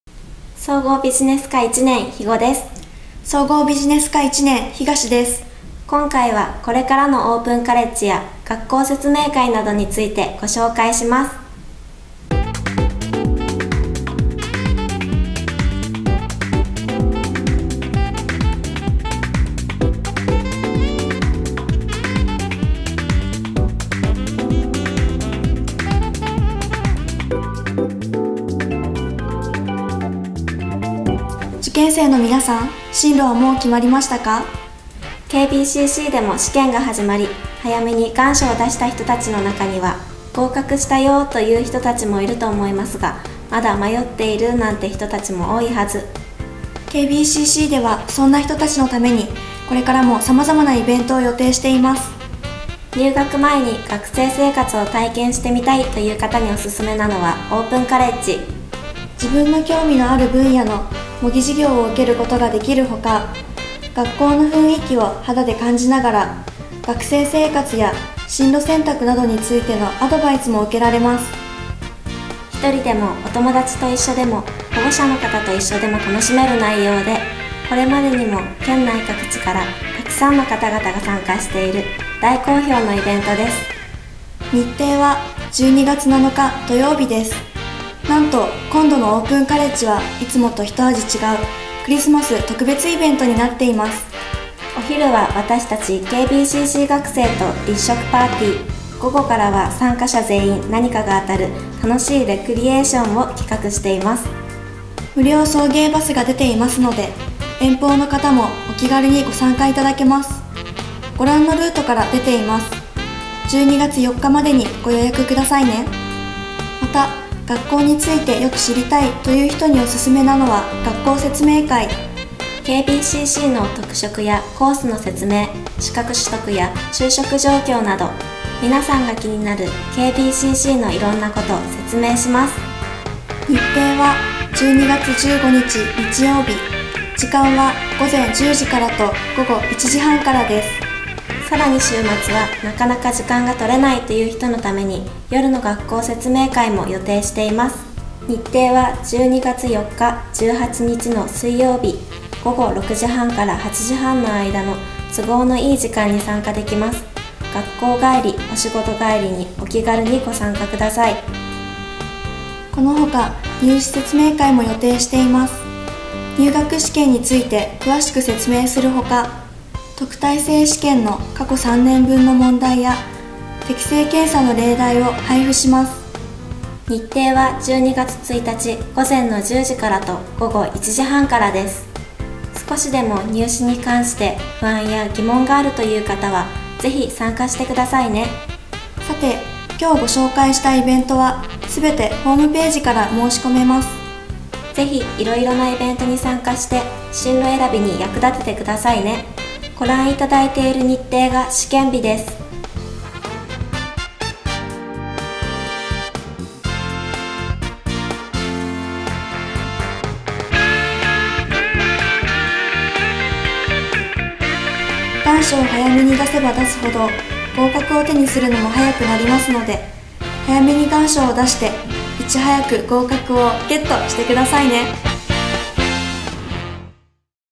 KBCC-TVインフォメーション(鹿児島 専門学校)
鹿児島の専門学校 KBCC　鹿児島情報ビジネス専門学校のオープンカレッジや学校説明会などのイベント情報。
学校の紹介をKBCC-TV学生レポーターがお伝えします。